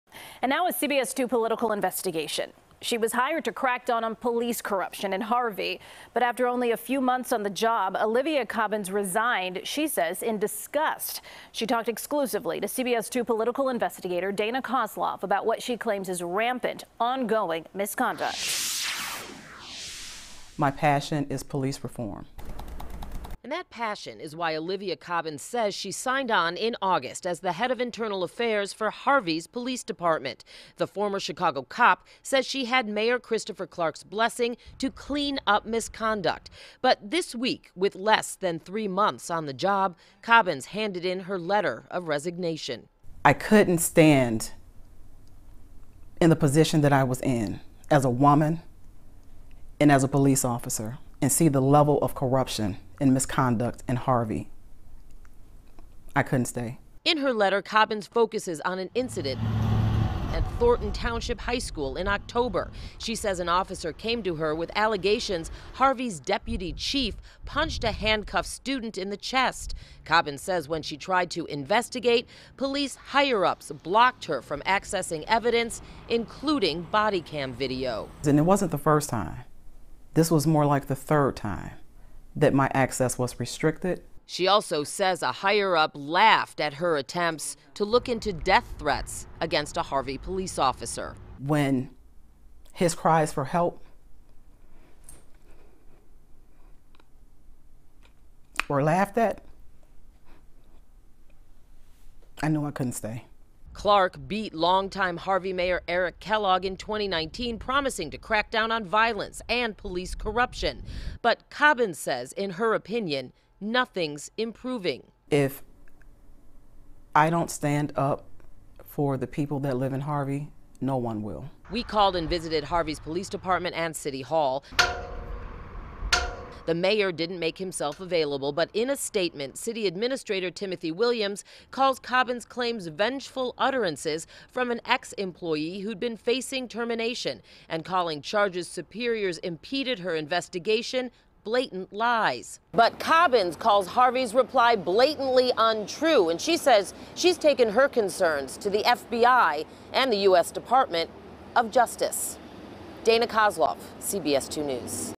Head Of Internal Affairs For Harvey Police Came To Crack Down On Corruption, And Now She Has Quit
Olivia Cobbins was hired to crack down on police corruption in Harvey – but only after a few months on the job, she resigned in disgust. CBS 2 Political Investigator Dana Kozlov talked with Cobbins exclusively.